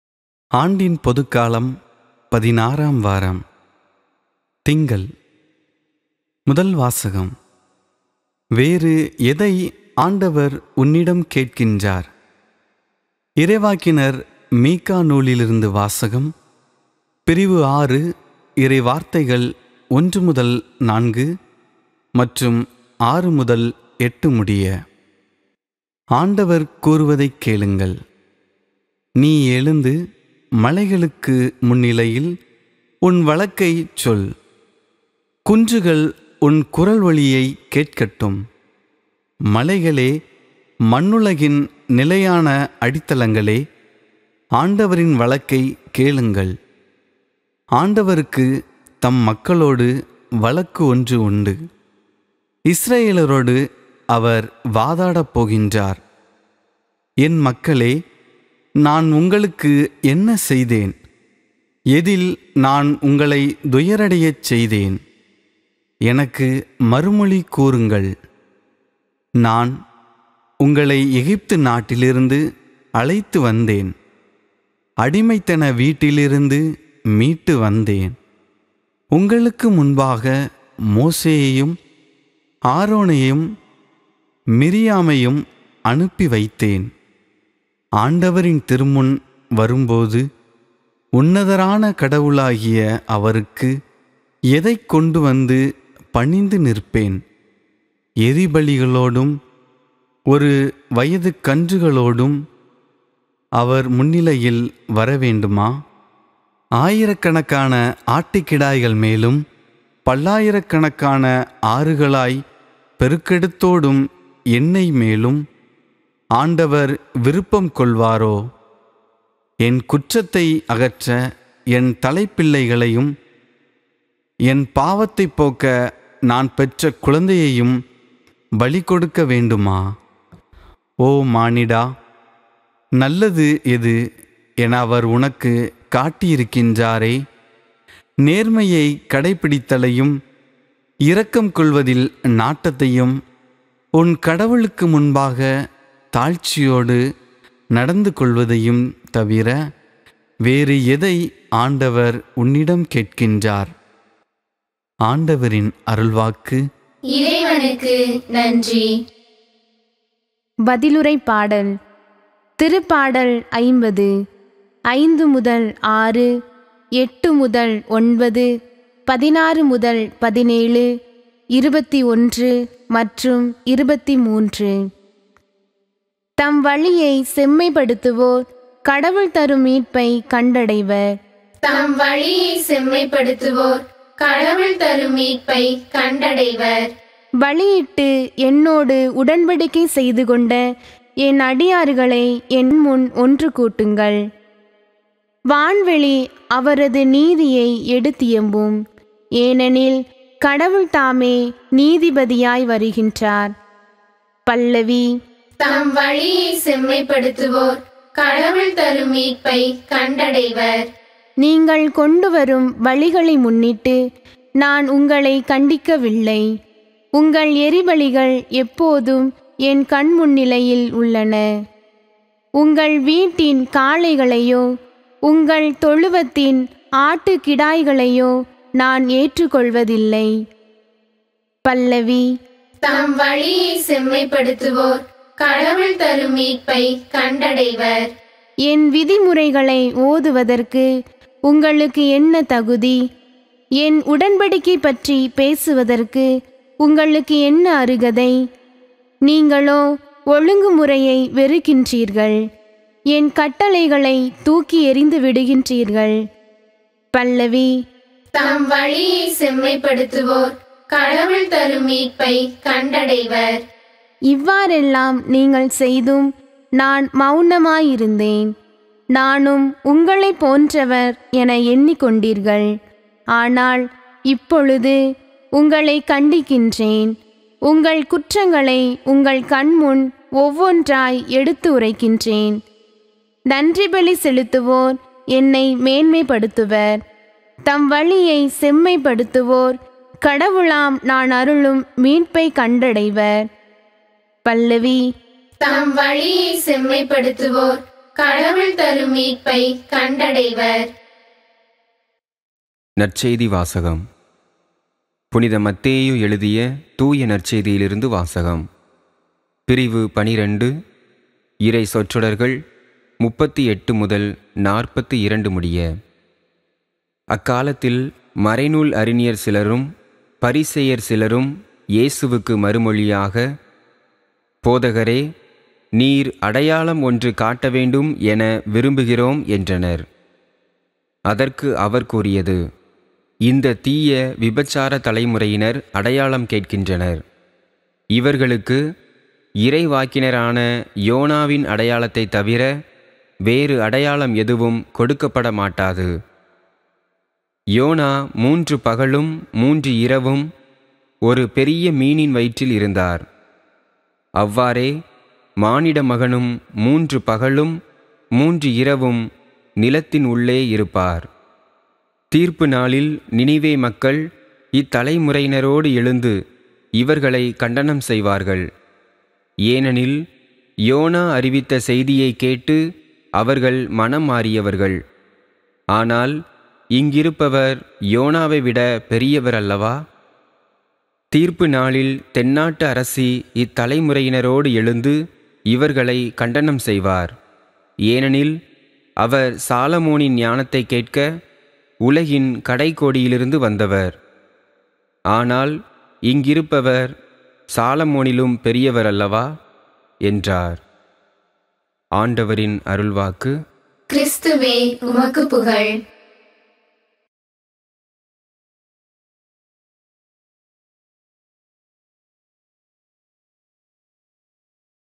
பொதுக்காலம் 16ஆம் வாரம் திங்கள் | திருப்பலி வாசகங்கள் | Daily Readings | Monday 18 07 2022
00:00 - முதல் வாசகம்
02:39 - பதிலுரைப்பாடல்
05:02 - நற்செய்தி வாசகம்